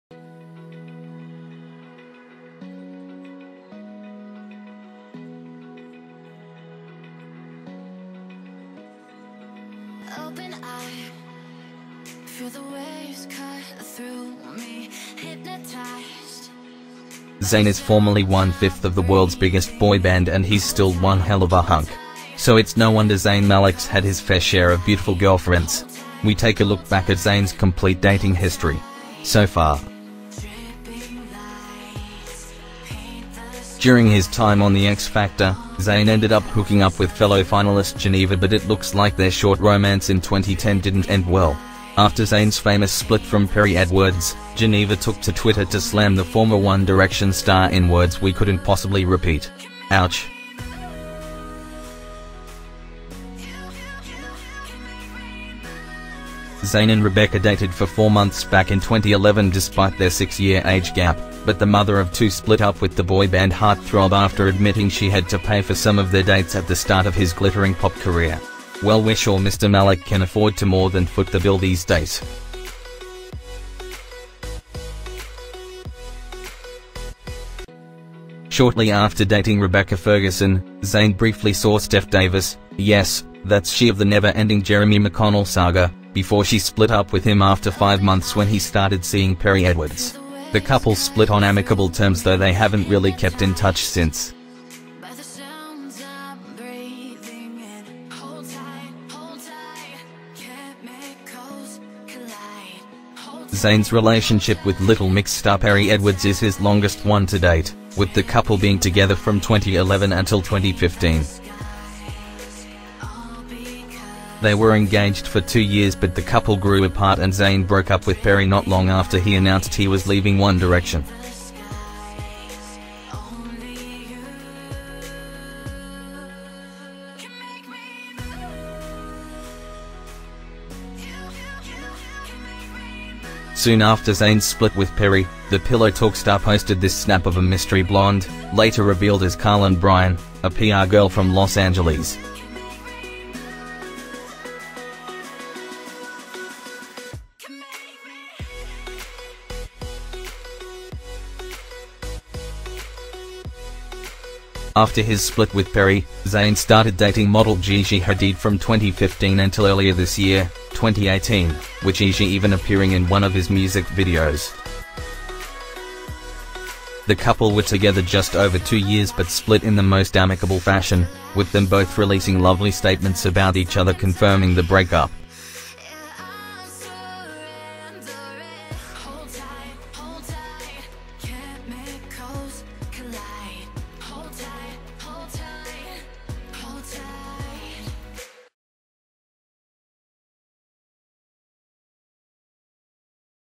Zayn Malik and Gigi Hadid I Geneva Lane I Perrie Edwards I Perrie Edwards I Rebecca Ferguson
Zayn is formerly 1/5th of the world's biggest boyband and he's still
one h... of a hunk... so it's no wonder Zayn Malik's had his fair
share of beautiful girlfriends! We take a look back at Zayn's complete dating history... so far!